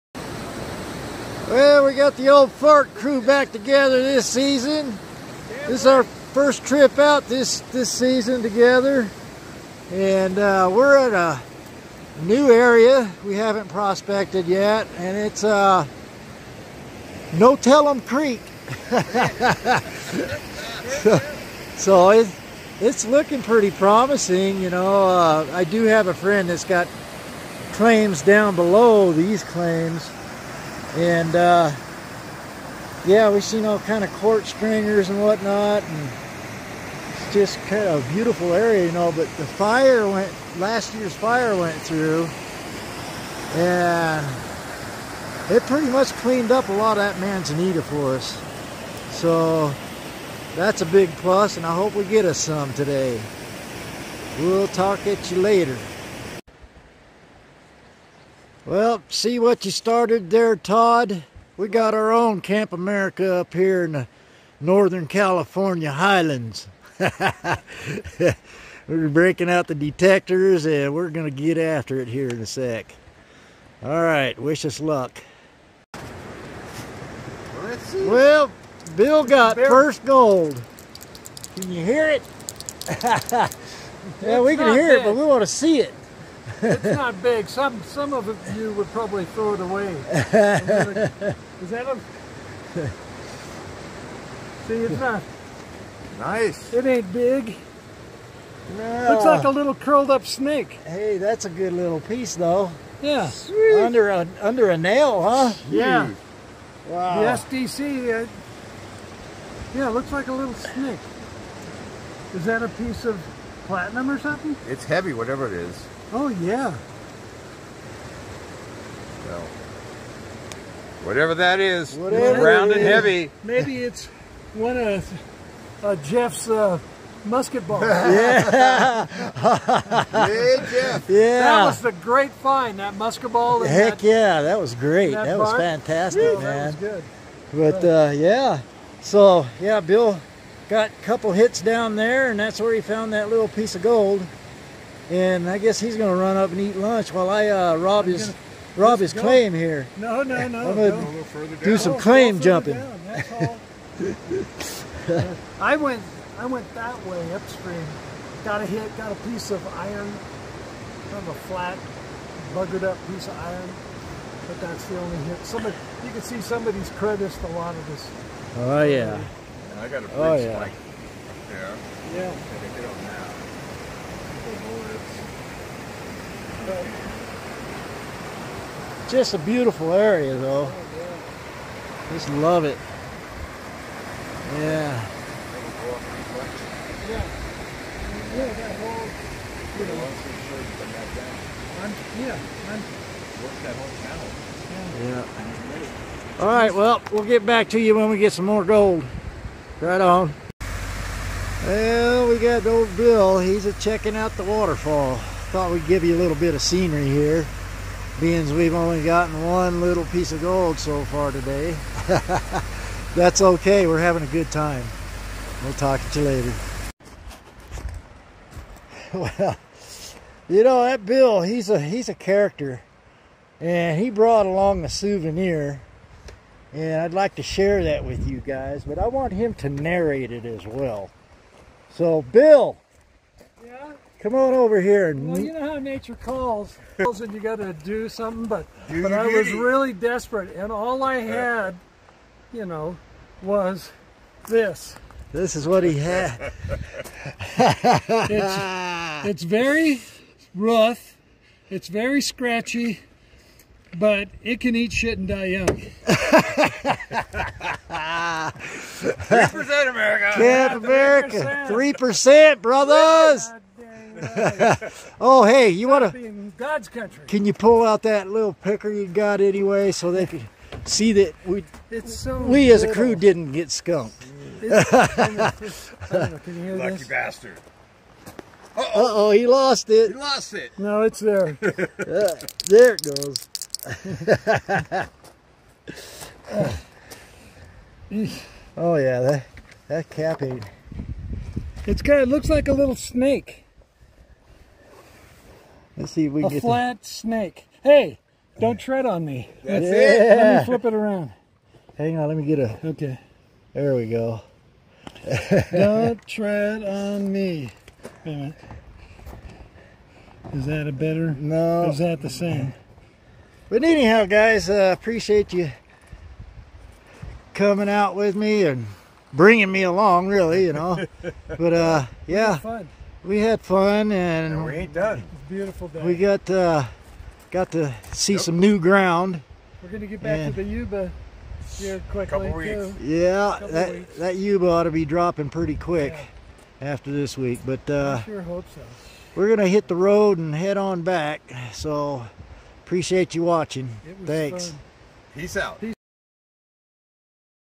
Bill, Devin & Geno Gold Exploration
Prospecting is a lot of fun with Bill and Devin. Gold is where you find it and im so glad I found them. Friendship is priceless.